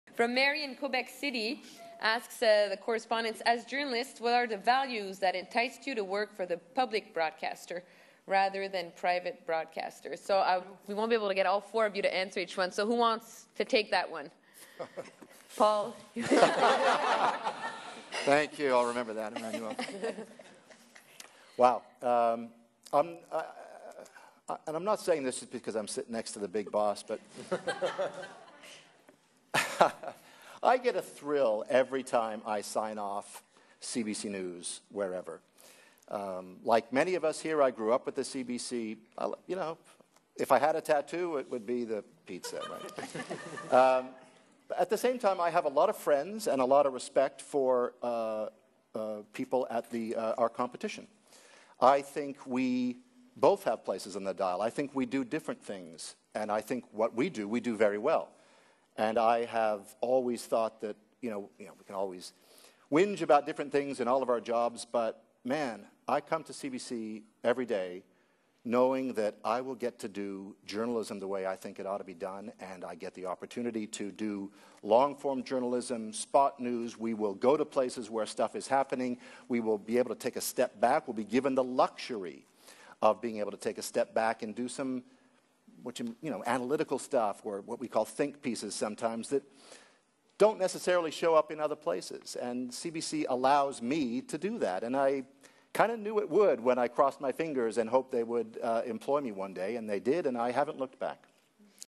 Paul Hunter - Annual Public Meeting
Question from Mary, QC: As journalists, what are the values that enticed you to work for Canada's public broadcaster?